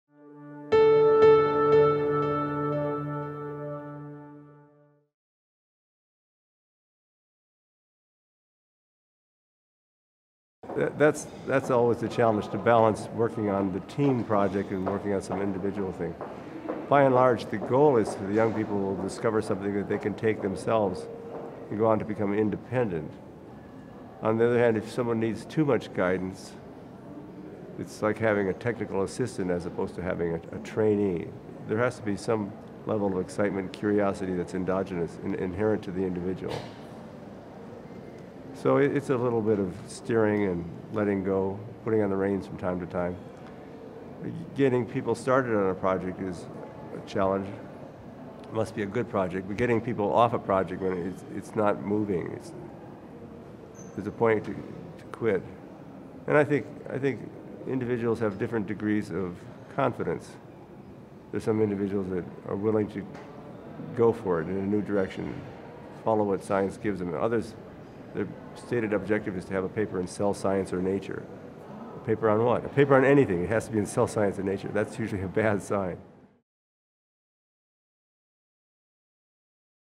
How do you encourage your students to become independent scientists? Peter Agre
Peter Agre, awarded the 2003 Nobel Prize in Chemistry, visited Moscow State University as part of the Nobel Prize Inspiration Initiative. Through the Initiative, Nobel Laureates give career advice for young scientists, explain their discoveries and give insights into life after the Nobel Prize.